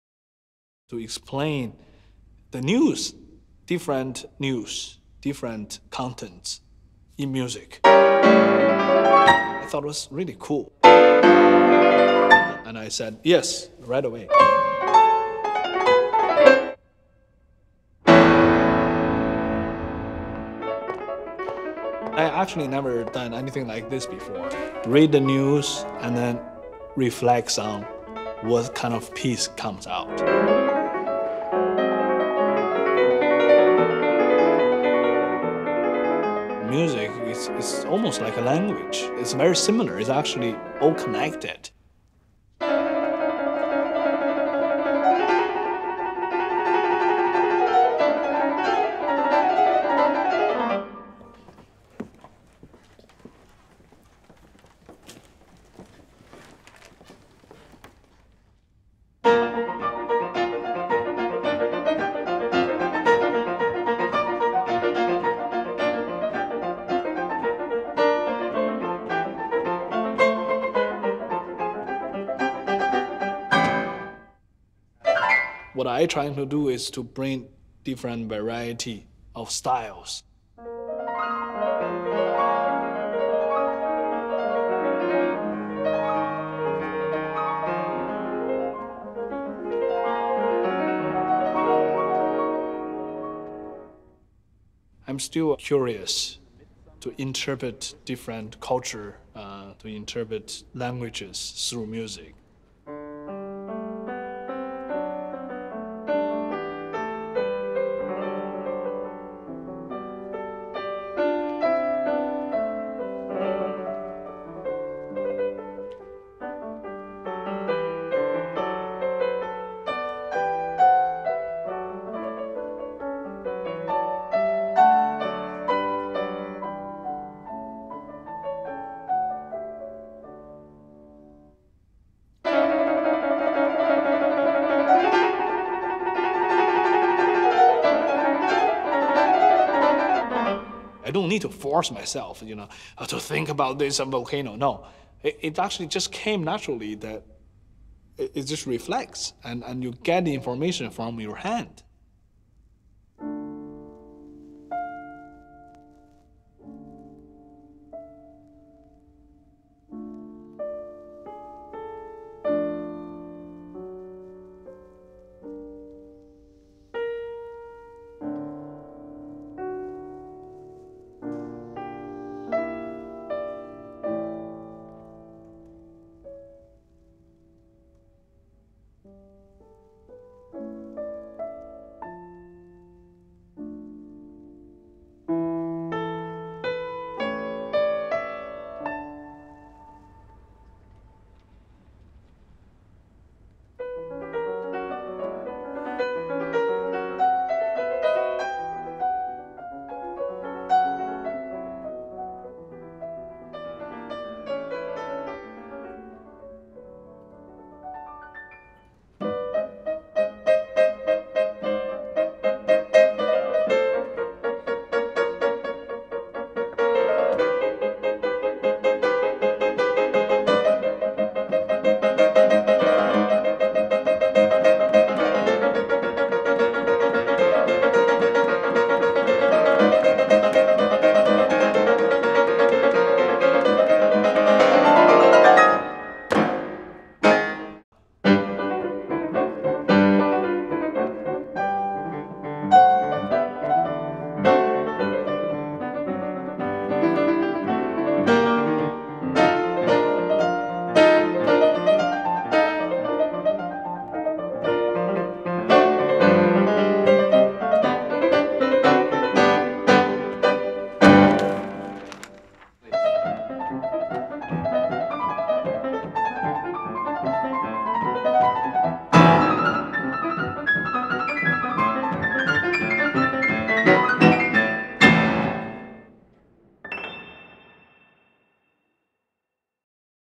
Kluger Kopf: Lang Lang spielt die F.A.Z.
Die legendäre Anzeigenserie der Frankfurter Allgemeinen Zeitung und Scholz & Friends „Dahinter steckt immer ein kluger Kopf“ feiert ihr 20-jähriges Jubiläum. Mit einem besonderen Höhepunkt: Weltstar Lang Lang interpretiert die Frankfurter Allgemeine Zeitung auf seine ganz eigene Weise.